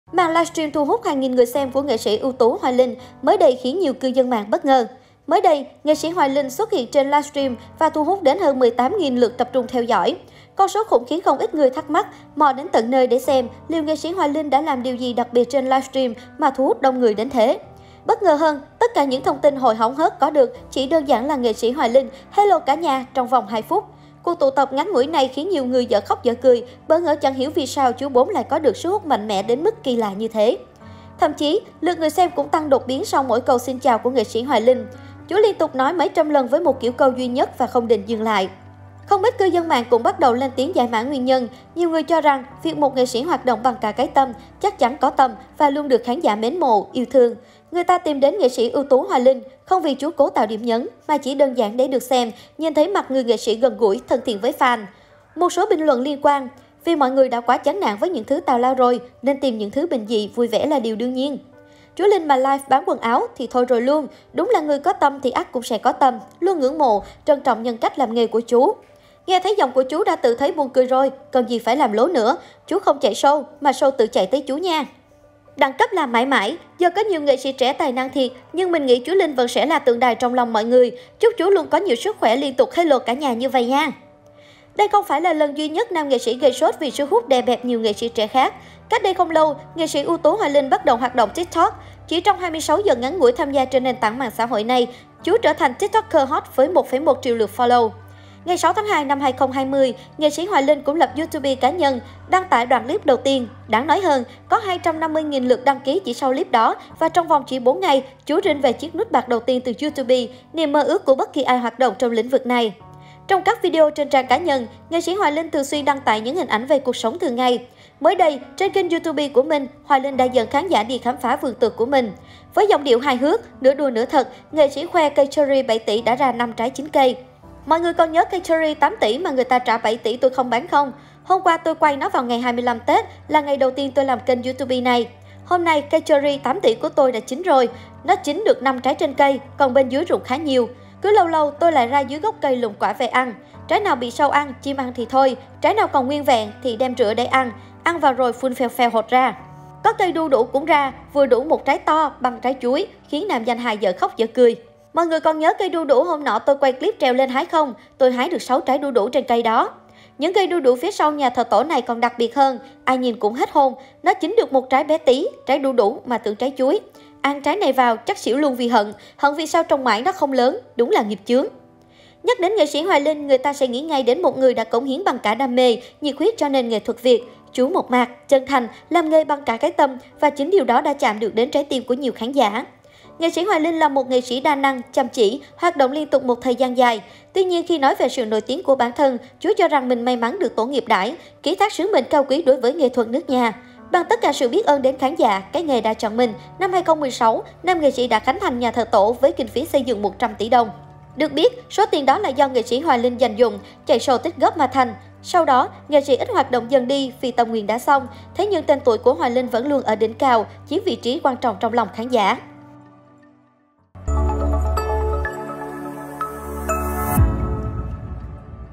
NS Hoài Linh cực hot: 2 phút nói "Hello cả nhà” 18 ngàn người xem
Màn livestream thu hút hàng nghìn người xem của NSƯT Hoài Linh mới đây khiến nhiều cư dân mạng bất ngờ.
Mới đây, nghệ sĩ Hoài Linh xuất hiện trên livestream và thu hút đến hơn 18.000 người tập trung theo dõi. Con số “khủng” khiến không ít người thắc mắc, “mò” đến tận nơi để xem liệu nghệ sĩ Hoài Linh đã làm điều gì đặc biệt trên livestream mà thu hút đông người đến thế.